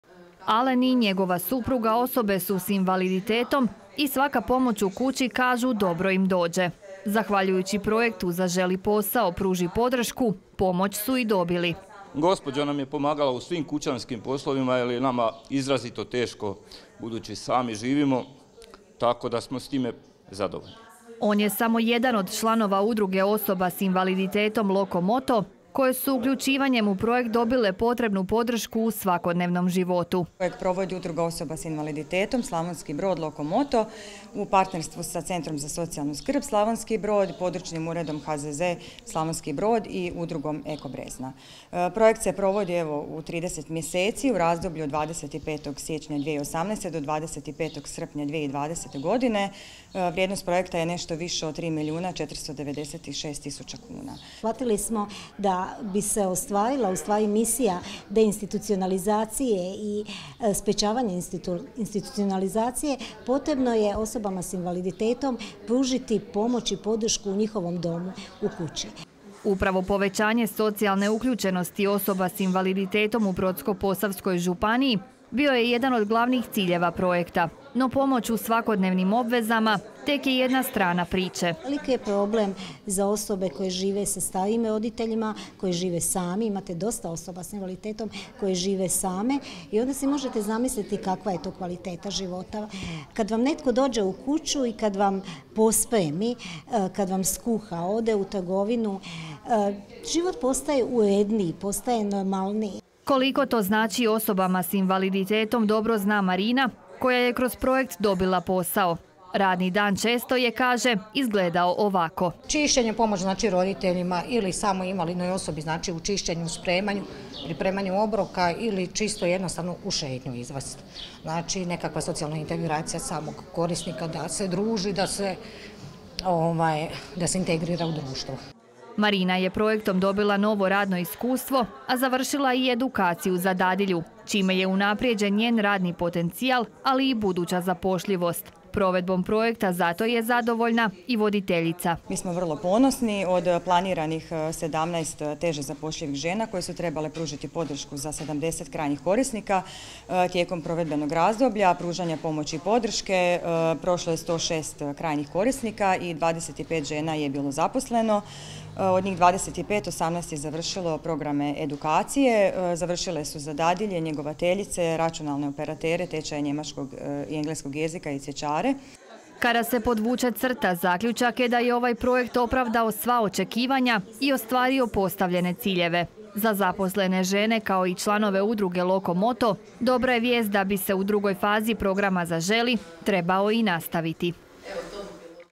SBTV - DNEVNIK - ZAVRŠAVA PROJEKT ZAŽELI U UDRUZI "LOCO-MOTO" - 02.07.2020.
Projektu "Zaželi posao – pruži podršku" Udruge "Loco-Moto", bliži se kraj. Završna konferencija  održana je danas, a tom prilikom mogla su se čuti iskustva i dojmovi korisnika projekta kao i žena koje su zahvaljujući njemu dobile posao. Koliko su zadovonji, pogledajte u priči koja slijedi.

DNEVNIK SBTV-a | PON-PET | 20:00 i 22:00